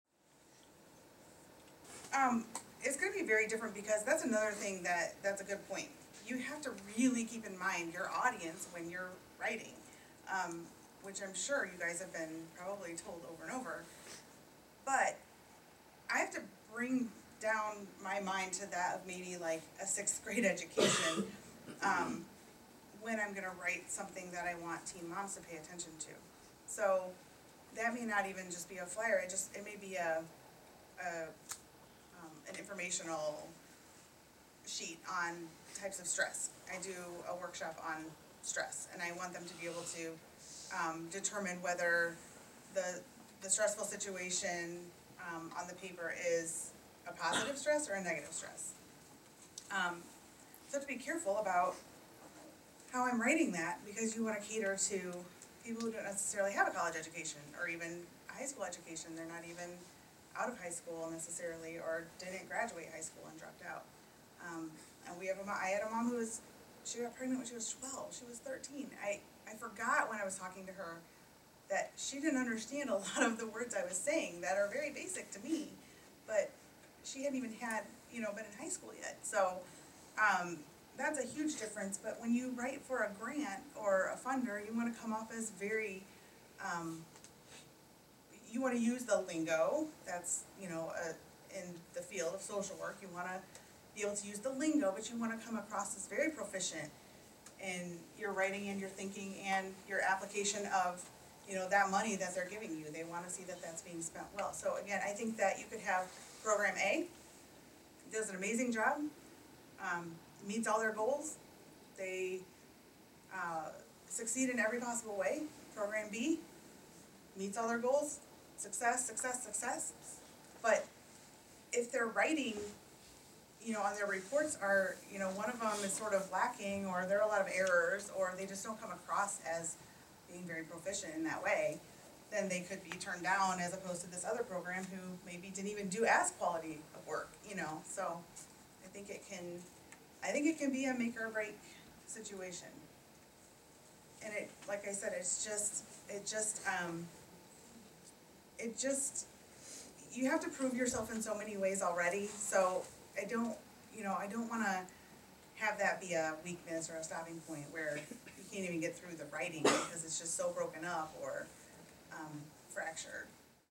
Make or Break Situation: Proofing Yourself
Jenn stresses the importance of keeping your audience in mind and how navigating very different writing situations requires you to better understand not only the requirements of the genre, but also the importance of being versatile and adapting your practices accordingly.